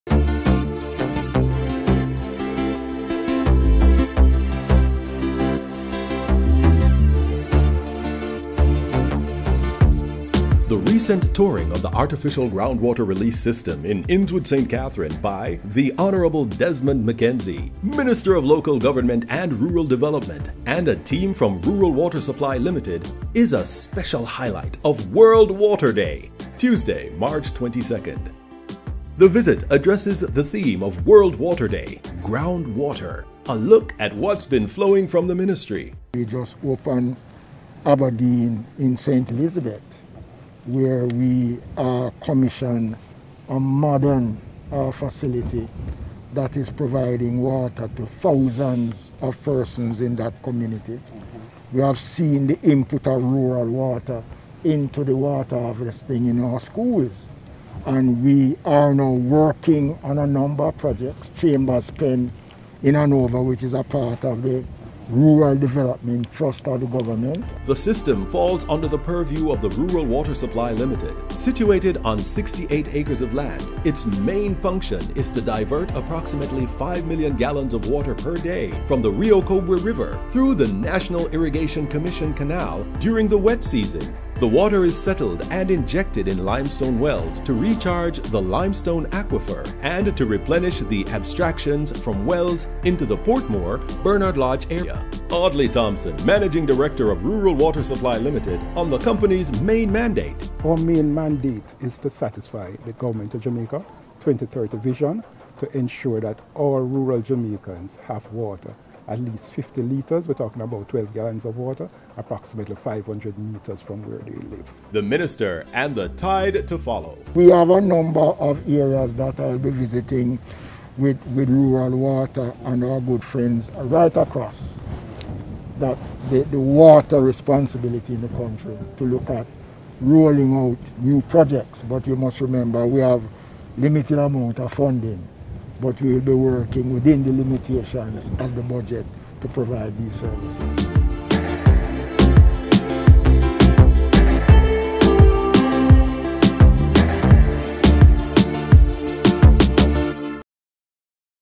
World Water Day 2022 - Tour of Artificial Groundwater Recharge System
Today is recognized as World Water Day and we commemorate the day under the theme 'Groundwater - making the invisible visible'. Here's a summary video of the Minister's Tour of the Underground Recharge Facility located at Innswood in St. Catherine, where he was given a first-hand look at how they divert approximately five (5) million gallons of water per day from the Rio Cobre River through the National Irrigation Commission (NIC) Canal during the wet season.